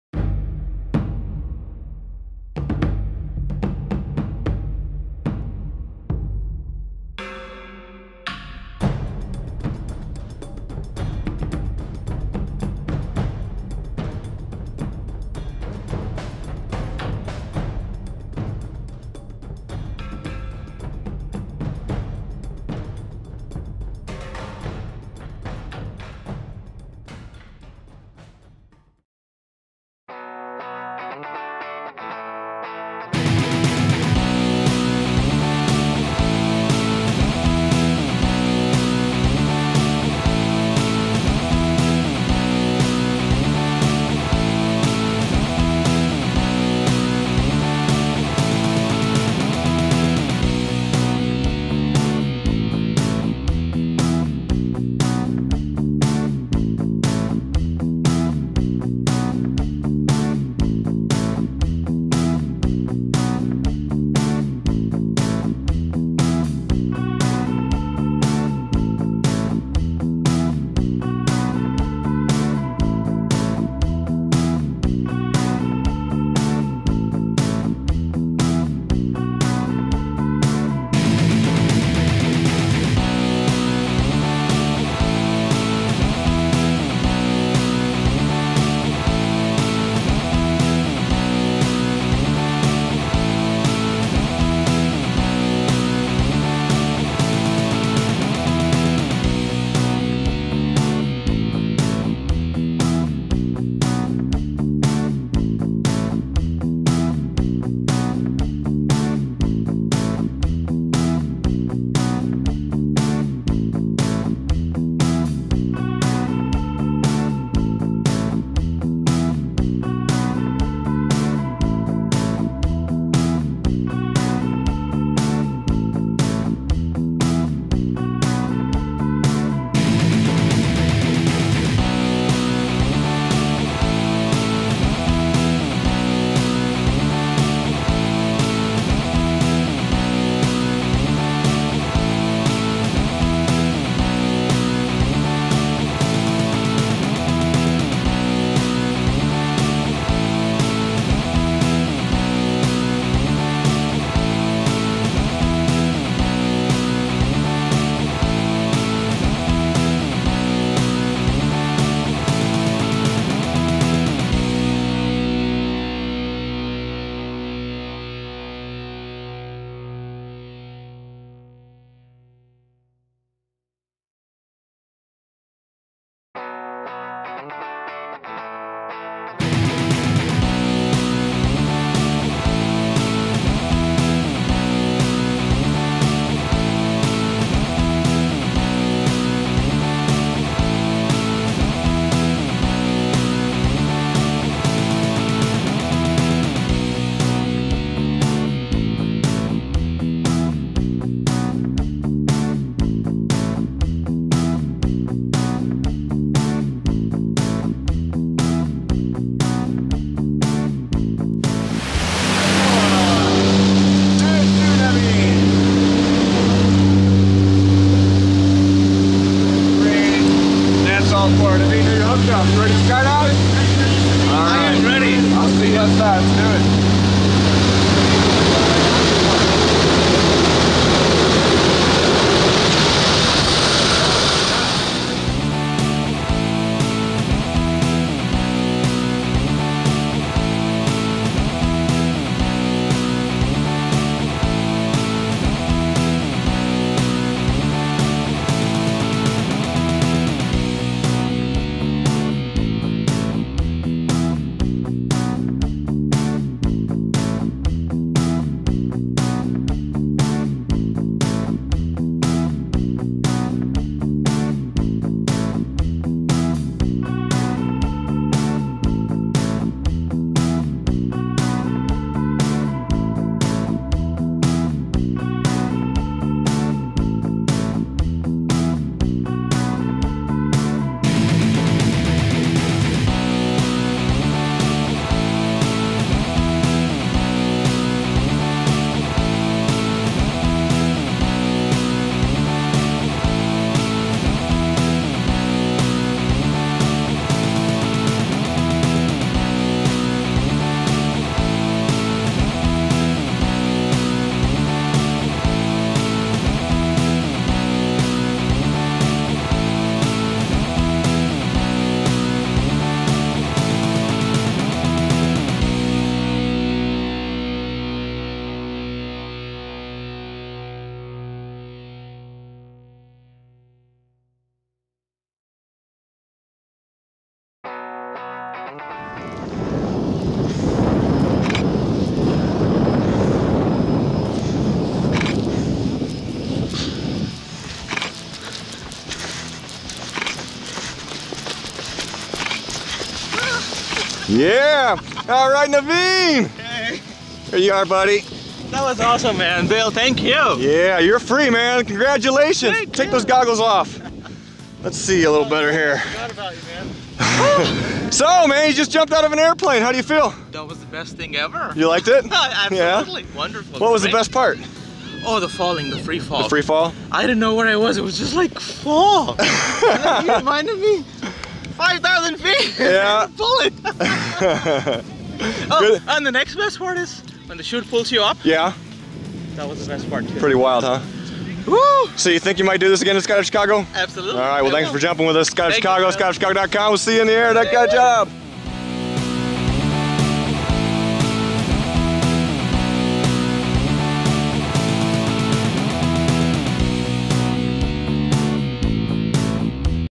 Naveen Bekkam's Tandem skydive!
Naveen Bekkam's tandem skydive on Sunday, June 17, 2012 with Will Pesek.